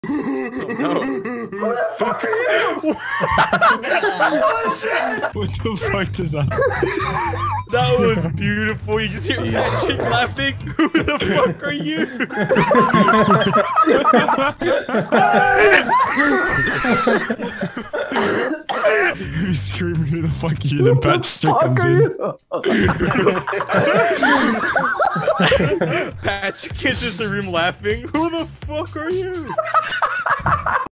WHO ARE YOU!!?
He enters, his identity is questioned, hilarity ensues